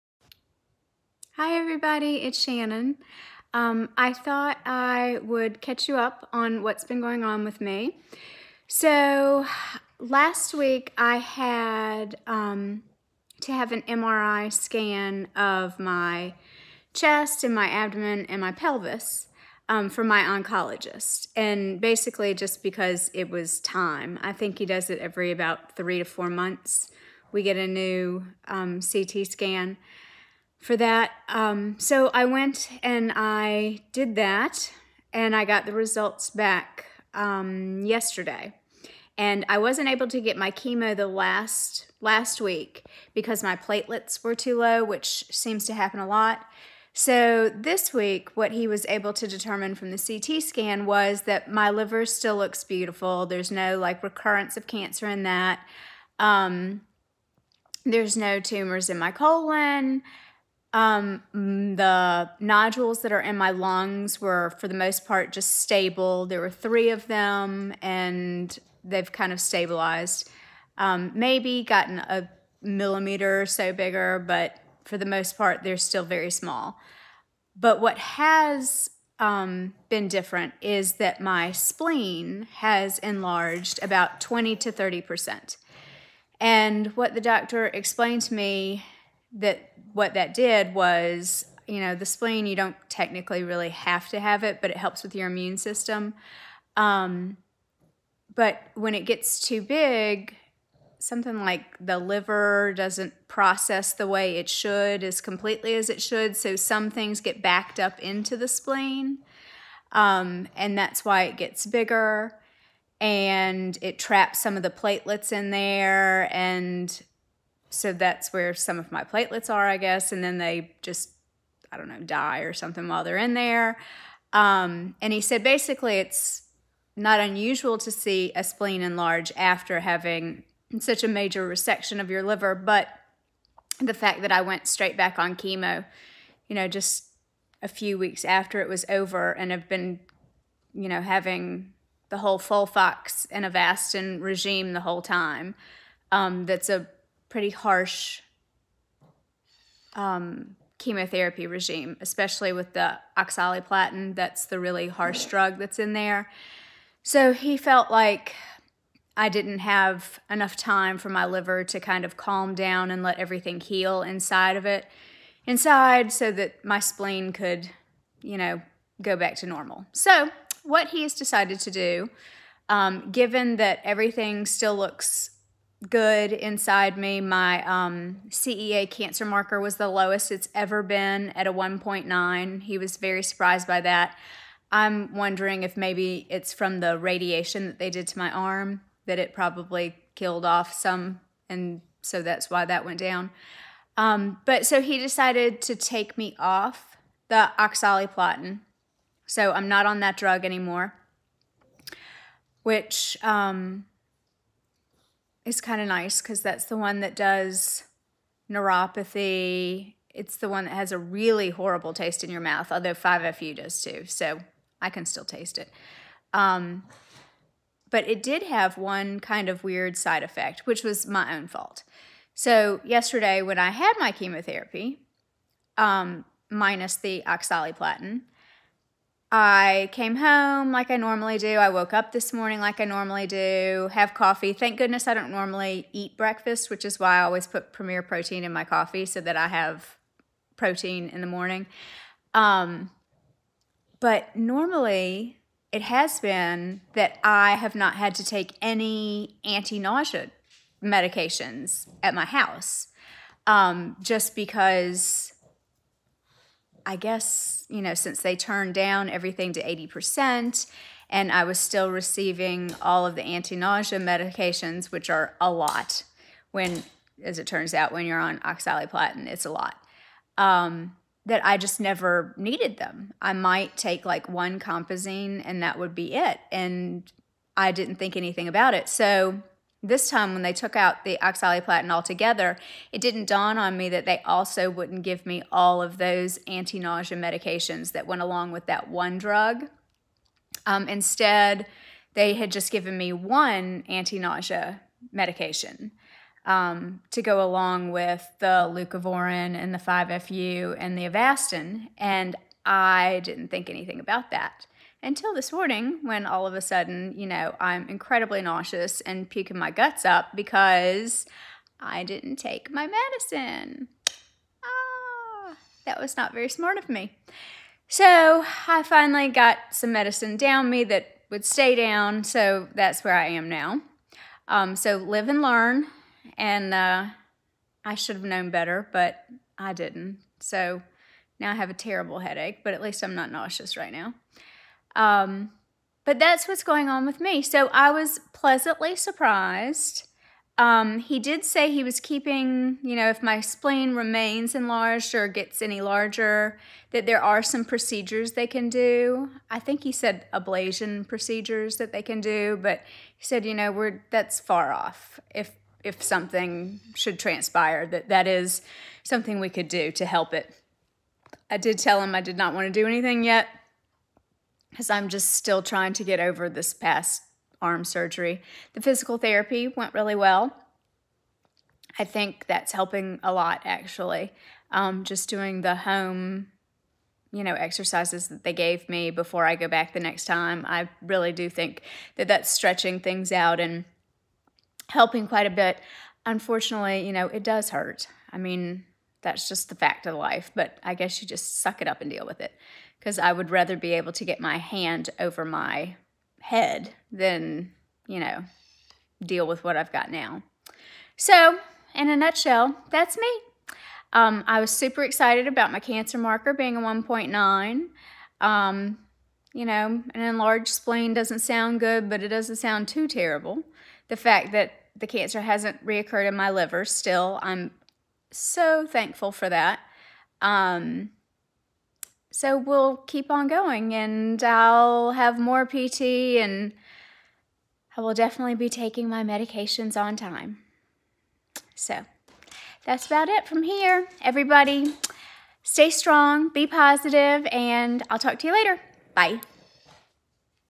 Cancer update and Different Chemo
I got back the results of my latest CT scan and CEA cancer marker.